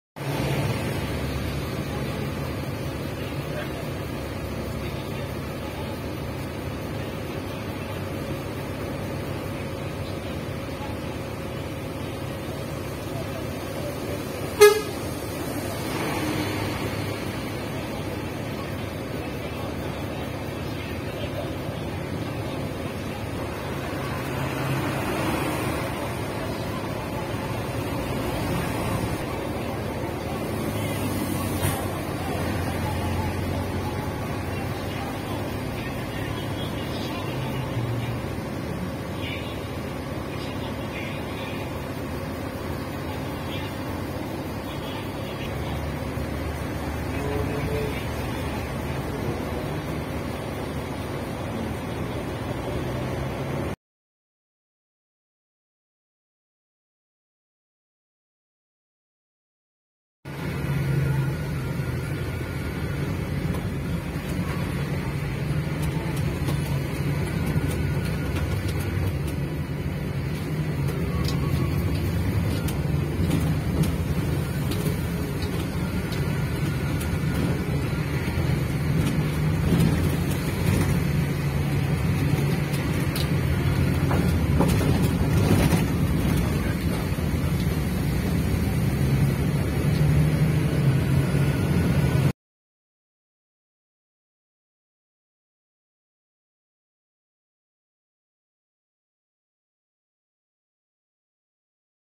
Cruzando Las Lajas (Pajarito - Aguazul) 20/04/2024
Cruzando el sector Las lajas hoy sábado Vía Pajarito
Transversal del Cusiana (Abril 20/2024)